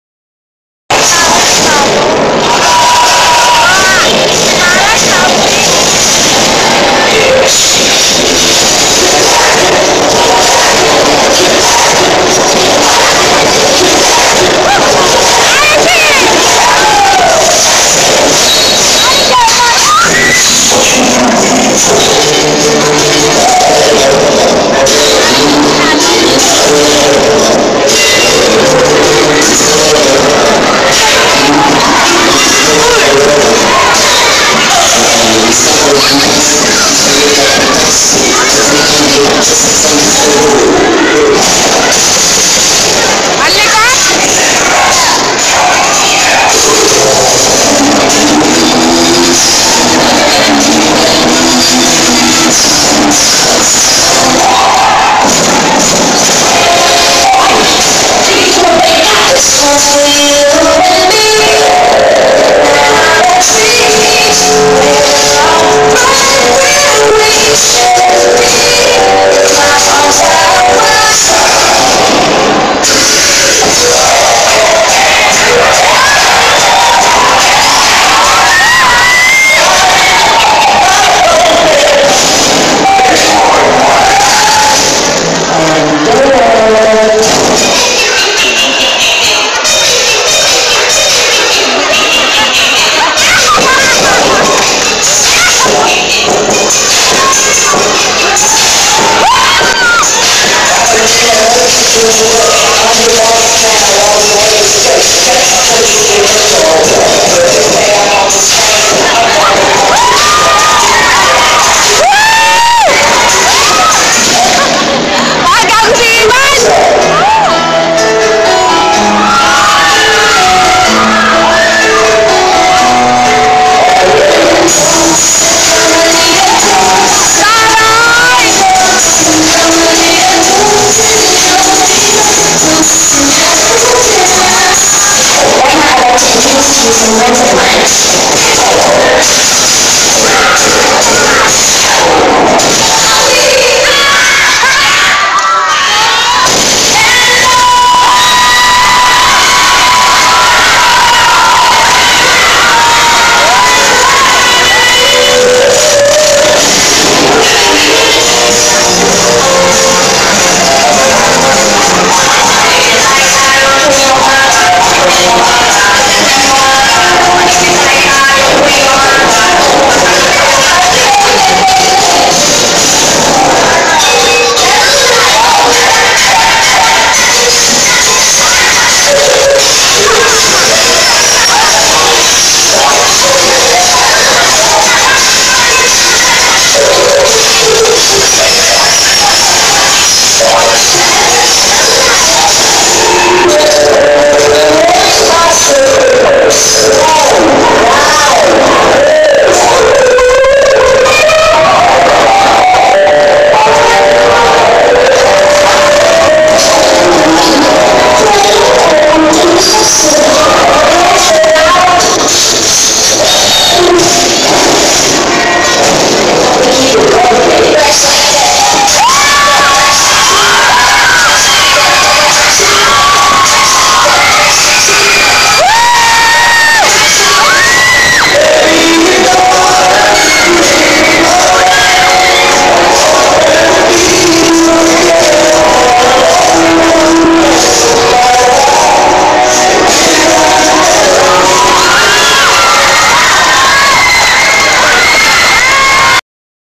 milkmaid dancers
mga batang walang magawa.daanin sa sayaw!!hahaha